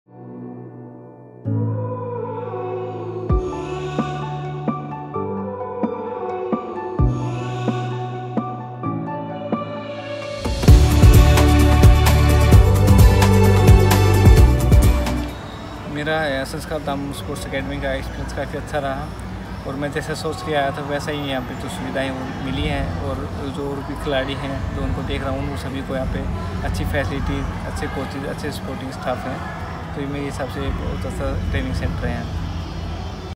Boxer Manish Kaushik at Sports Academy
Sanskardham welcomes Boxer Mr.Manish Kuashik, who visited us as a part of his rehabilitation program.
Born on 11th January 1996 in Bhiwani district, Manish Kaushik recently bagged a silver medal at the Commonwealth Games of 2018 and represented India in the Tokyo Olympics 2020.

We are incredibly grateful that he serves as a source of inspiration and astonishment for our aspiring athletes at Sanskardham Sports Academy.